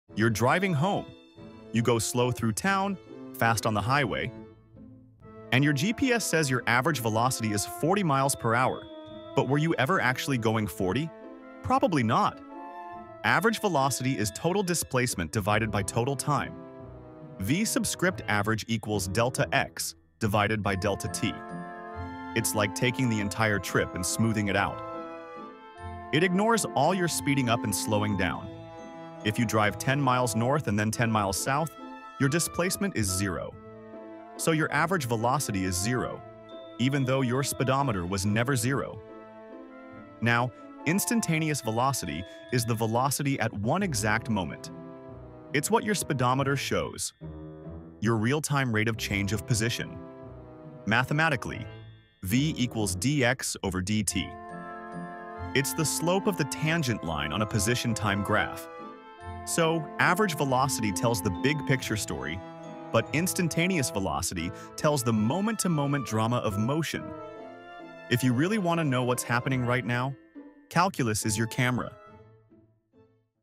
Why Average Velocity Lies to You
In this video, you will learn the difference between average and instantaneous velocity.

This video is apart of my Physics series.